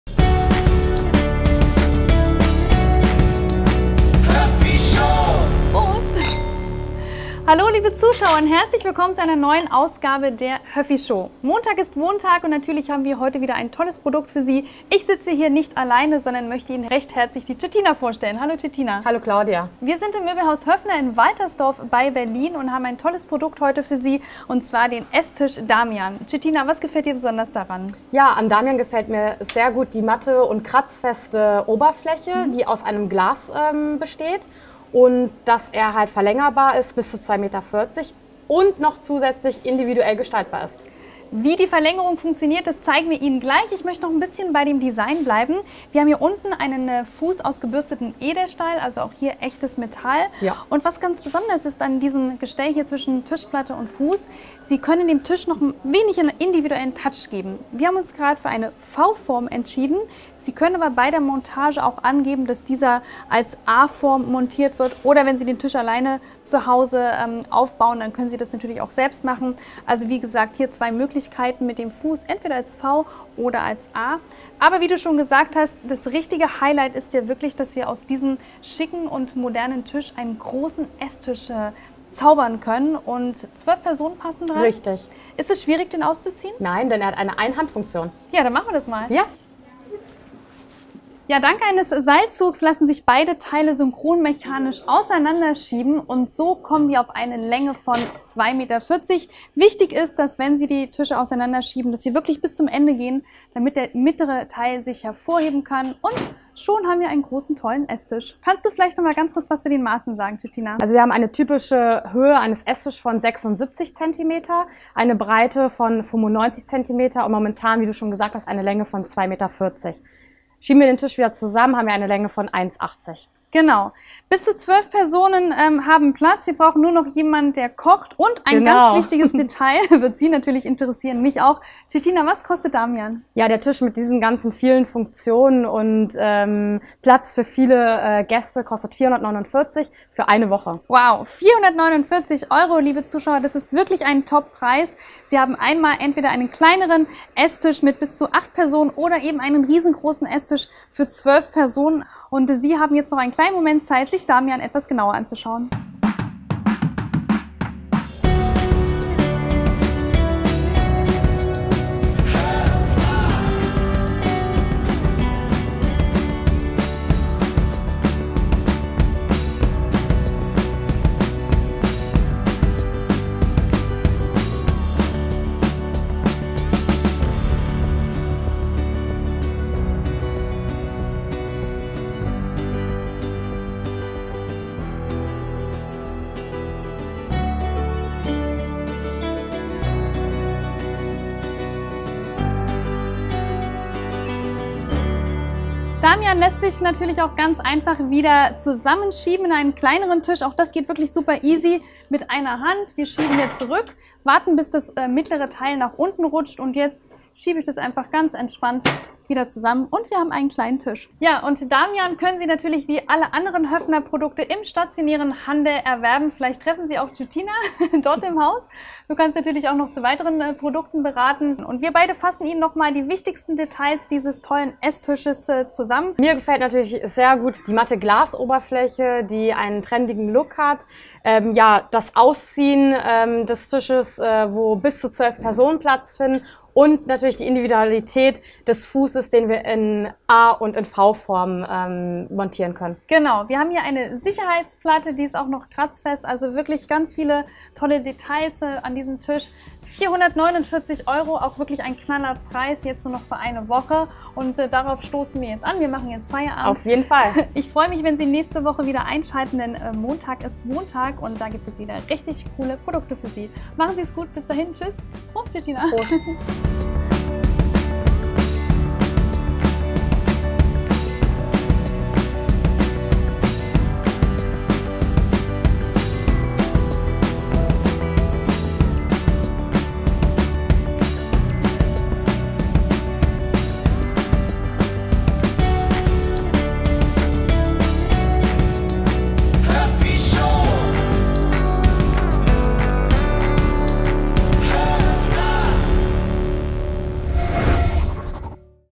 Die Höffi Show // Esstisch Damian
Damian ist ein ausziehbarer Esstisch. Dank des Synchronauszugs können Sie ihn bei Bedarf ganz einfach verlängern. Bei einer Länge von 240 Zentimetern finden bis zu 12 Personen Platz. Die Tischplatte ist aus Glas. Flecken können Sie so einfach wegwischen. Damian ist weiß – die Bodenplatte und Akzente in Edelstahloptik setzen sich ab. Übrigens: Sie können bei der Montage entscheiden ob die Gestell-Säule eine A- oder V-Form haben soll.